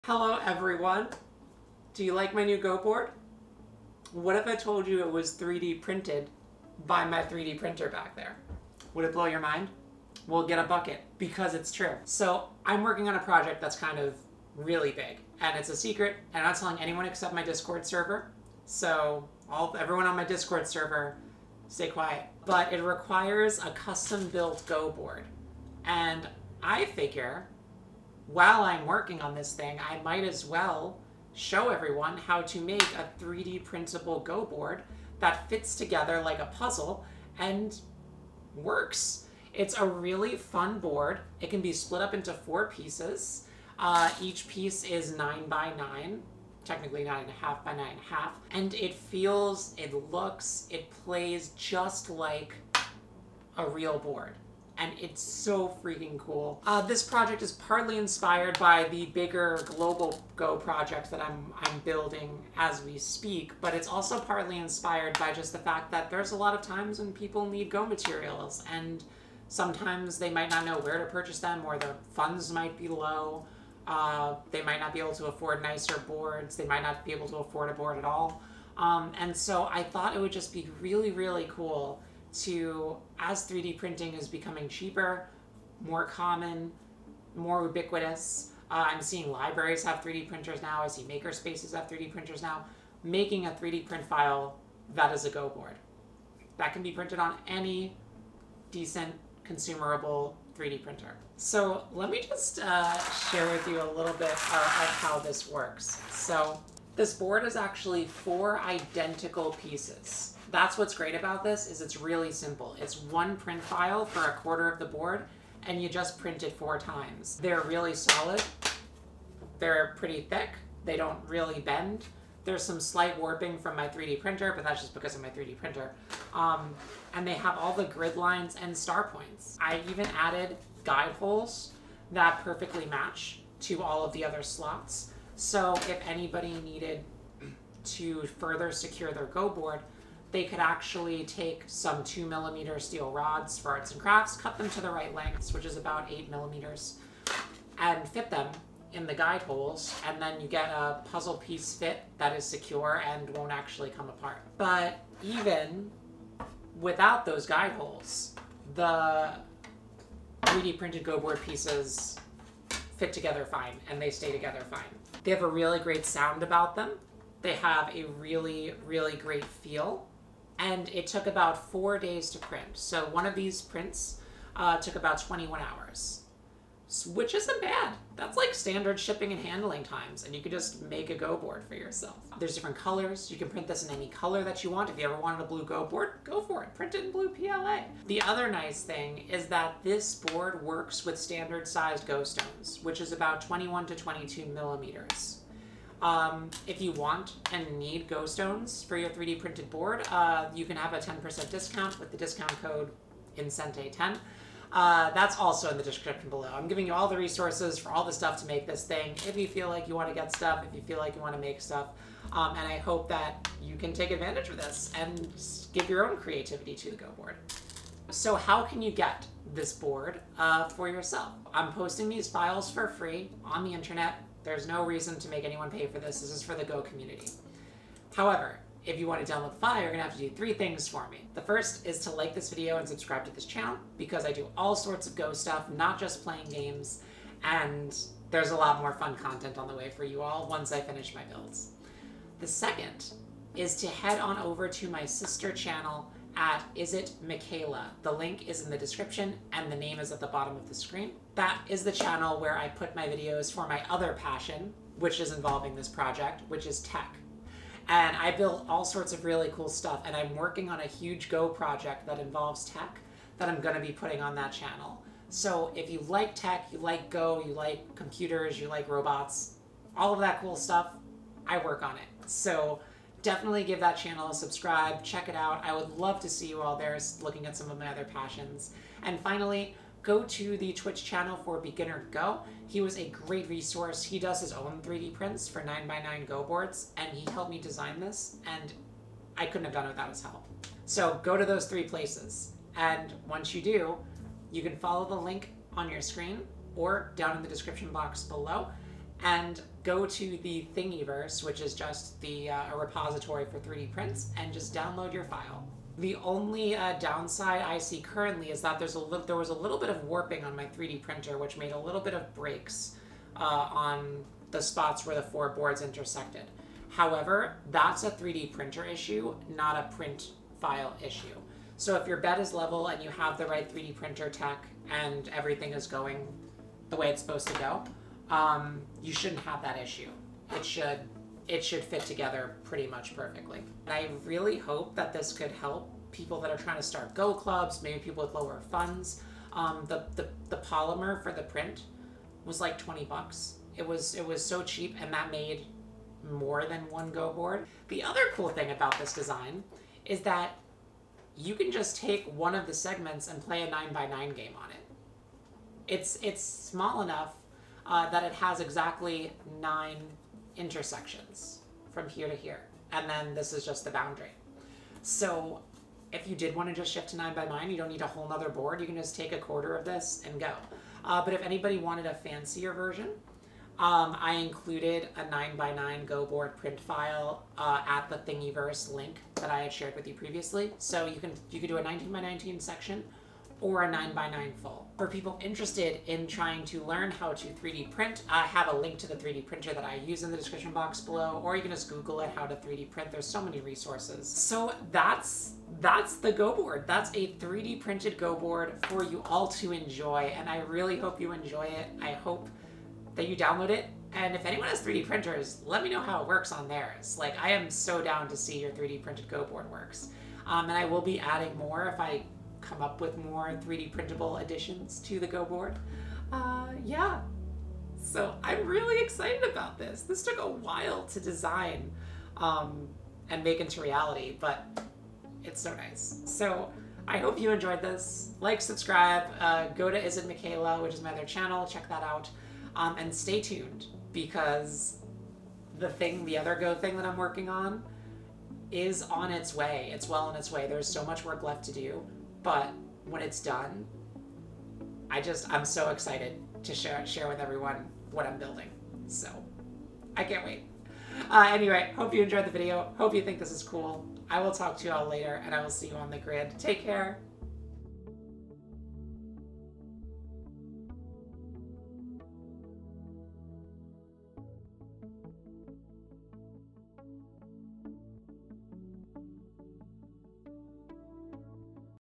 How To 3D Print Your Own Go Board!
All relevant links for files, materials, and resources are below - I hope you enjoy it and use this to spread Go to as many people as possible!

The 3D printer I used for this project:

The PLA I used for this project: